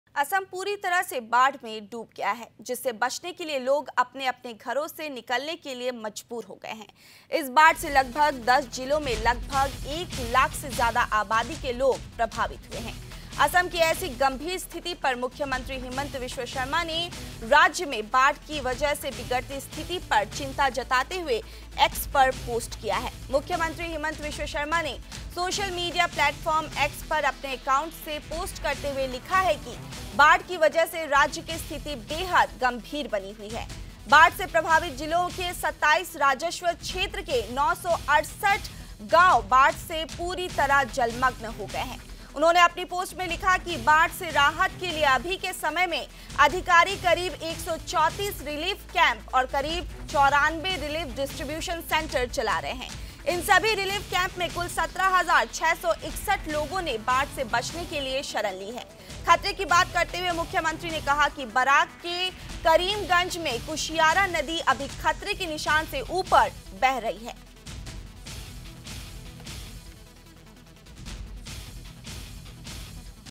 असम में बाढ़ का कहर…
असम में बाढ़ का कहर….बाढ़ से अब तक 39 की मौत …असम के 10 जिलों में करीब 1 लाख से ज्यादा लोग प्रभावित….मुख्यमंत्री हिमंत ने जताई चिंता…